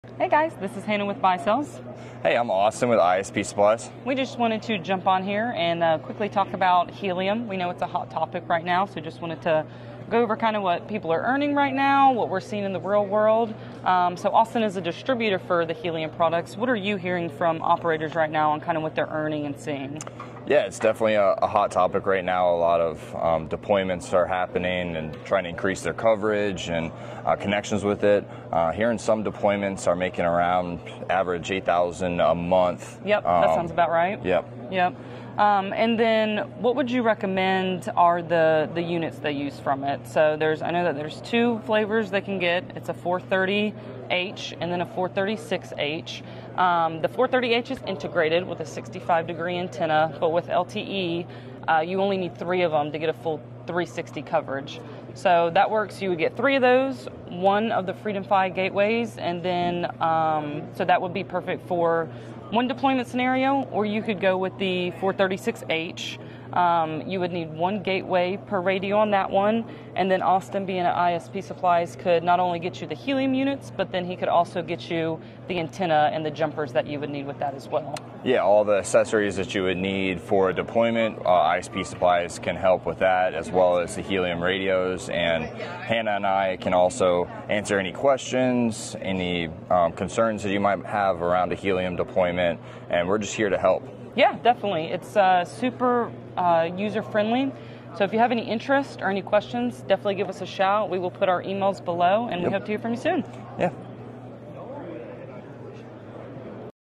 Baicells Helium discussion at Wispamerica 2024 (Oklahoma City, OK)
A discussion between Austin Parker (ISP Supplies), and Hannah Adamczyk (Baicells) on the Baicells Helium product.